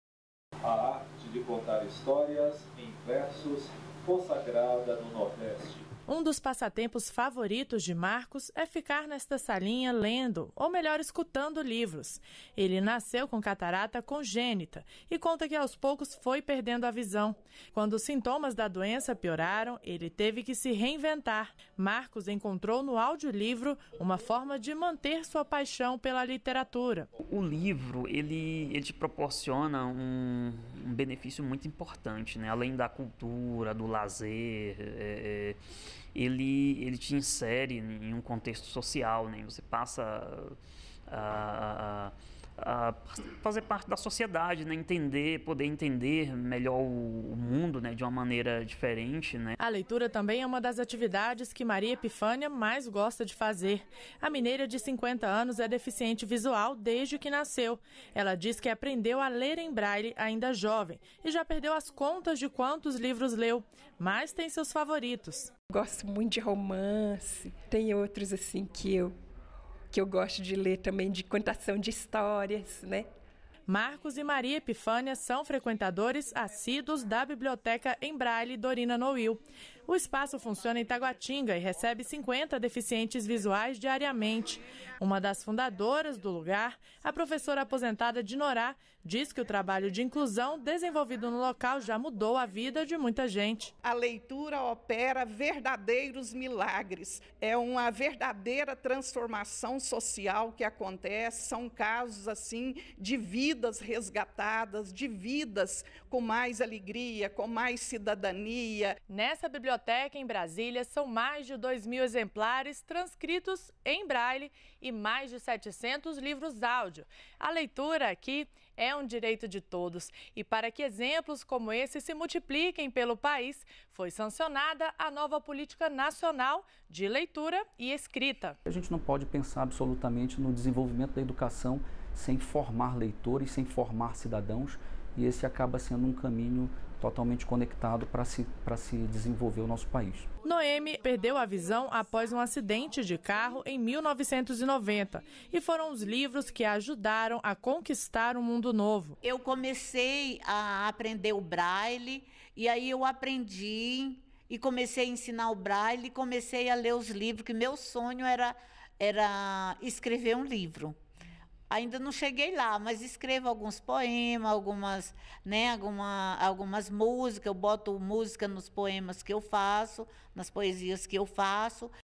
Nova Política de Leitura e Escrita é sancionada
REPÓRTER NBR - 20.07.18: leitura é um direito de todos. Para quem tem deficiência visual, uma forma de ver o mundo são os livros em braile e os áudio-livros.